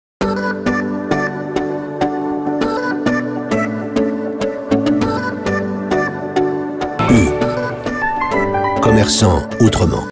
Black Friday 🔴⚫️
-22% de remise sur le sachet de frites rustiques au four - U jusqu'au 6 décembre 2022.